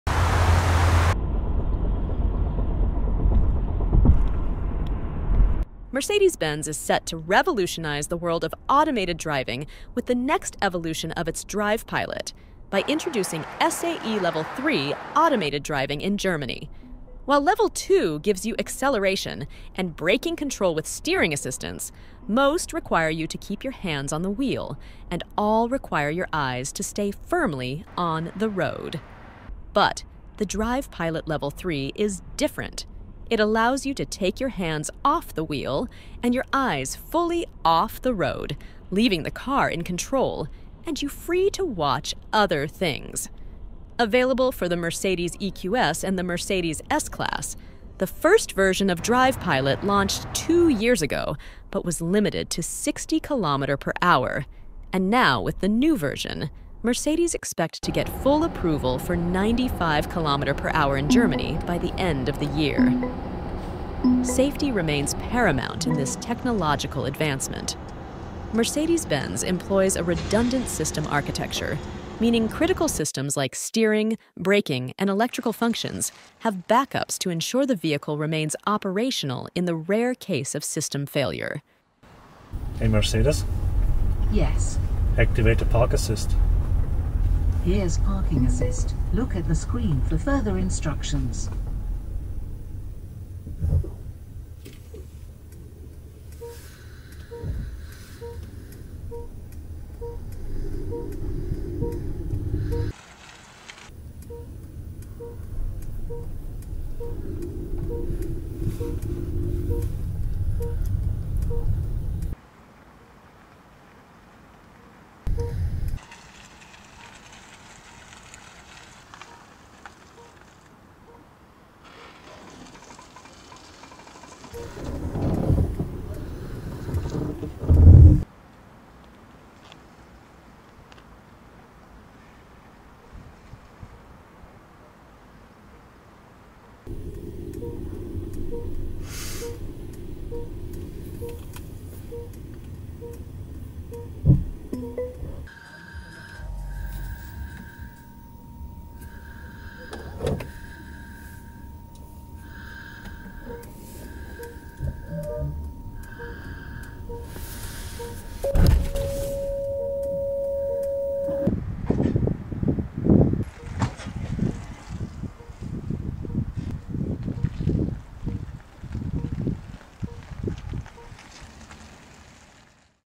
Mercedes-Benz Demonstrated 2025 Drive Pilot Level 3 Speed to 95 km/h
Mercedes-Benz is set to revolutionise the world of automated driving with the next evolution of its Drive Pilot, with introducing SAE Level 3 automated driving in Germany.
source : Mercedes